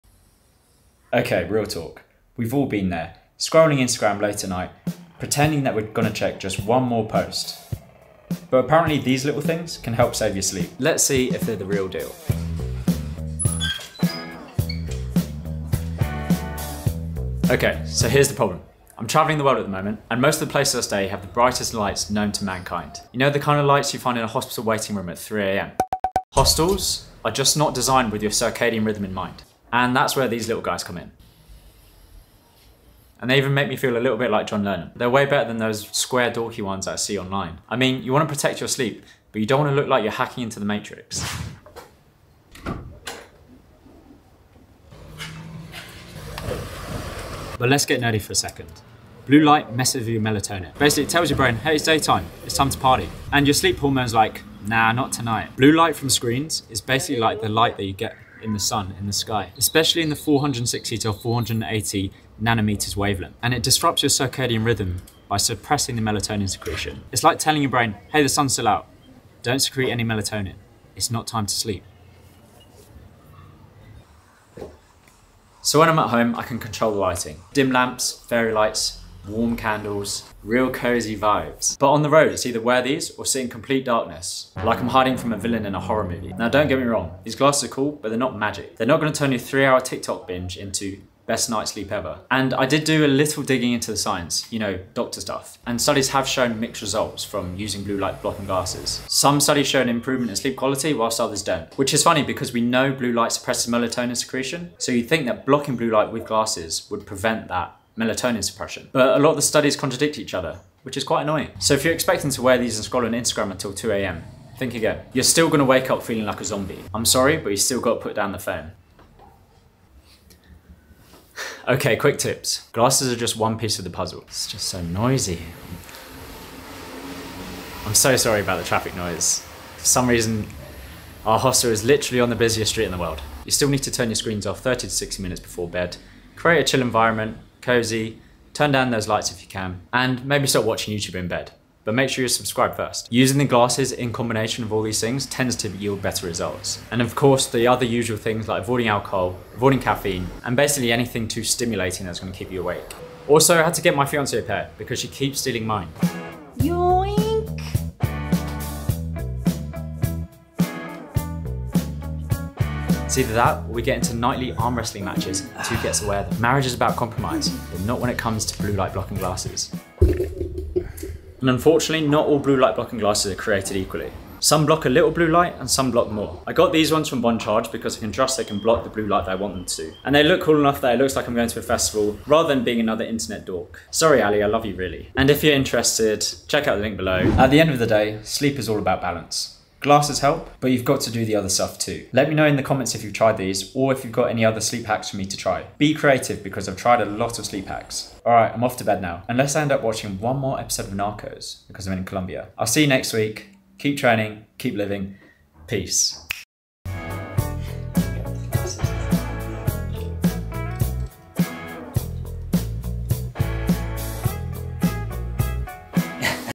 Do Blue Light Blocking Glasses Actually Help You Sleep?
I've been wearing blue light glasses at night to help me sleep this year, but are they the real deal? I'm staying in a variety of hostels/ hotels which have sub-optimal lighting for sleep. Basically too darn bright. I go over the simple science behind why they should work, mistakes you're making with blue light glasses at night and some more sleep tips.

STAMPS OF TIME:
00:01 intro
00:17 travel struggles with blue light
00:46 what blue light actually does
01:15 how i use them whilst travelling
01:51 what they don't do
02:00 practical tips for better sleep
02:33 my fiancé keeps stealing my glasses
02:51 choosing the right glasses
03:08 ending

This is Ep. 21/50 of 'Behind The Scenes Weekly' - a doctors career break vlog on how to live a healthier, happier life. See the full playlist here!